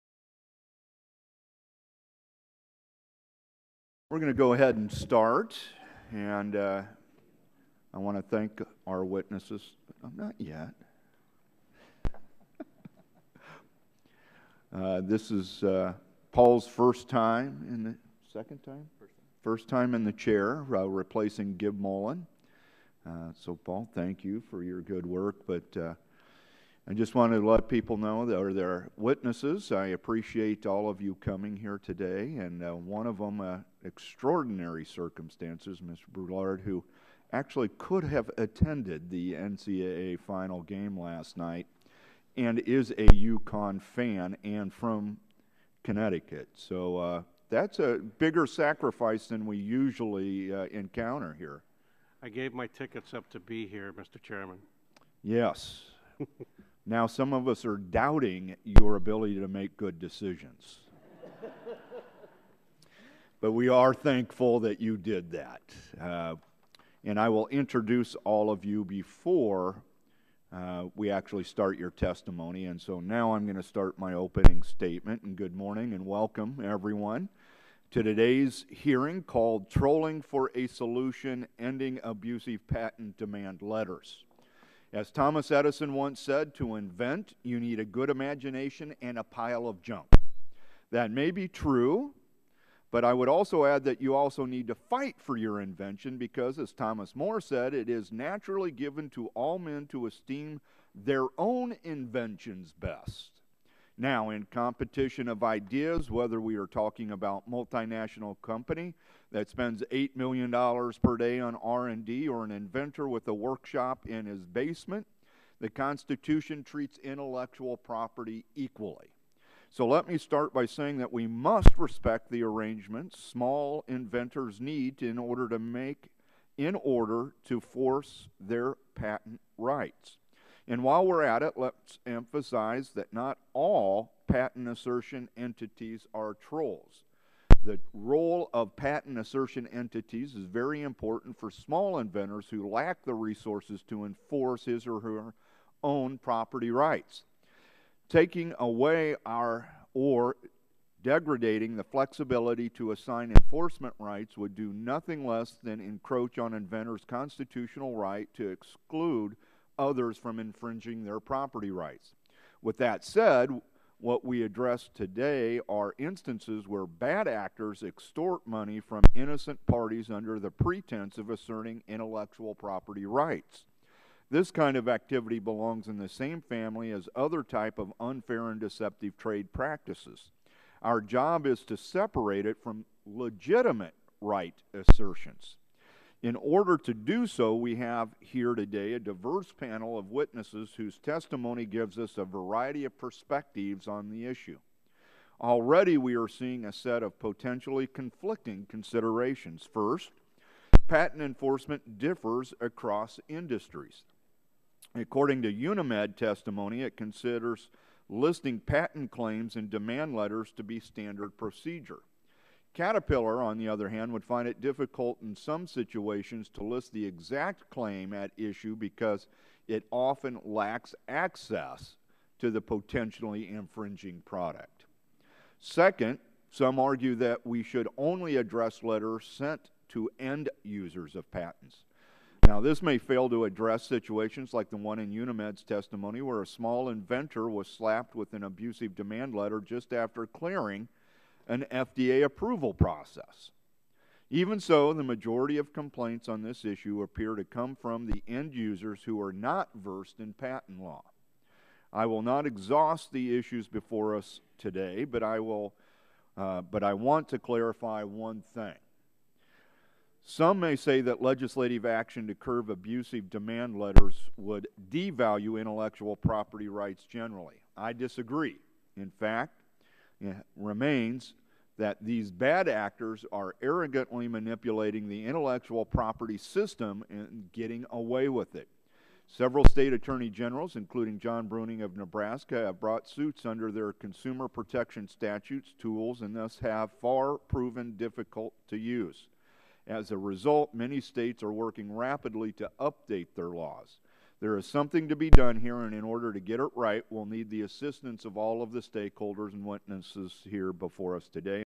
Subcommittee on Commerce, Manufacturing, and Trade Hearing
Rep. Lee Terry, Subcommittee Chairman, opens the hearing, "Trolling For a Solution: Ending Abusive Patent Demand Letters."